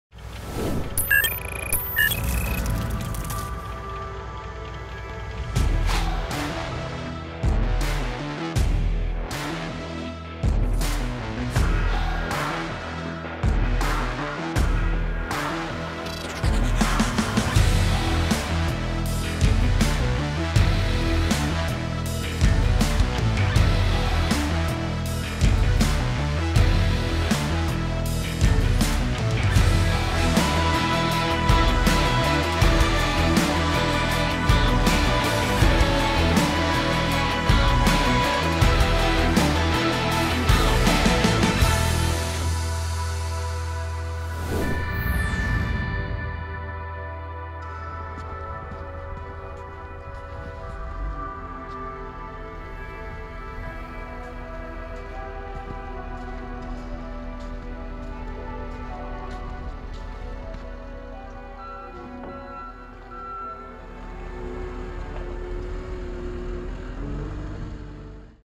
MUSKOX clears two driveways
Machine: John Deere 330G
Attachment: MUSKOX 22-78 Dually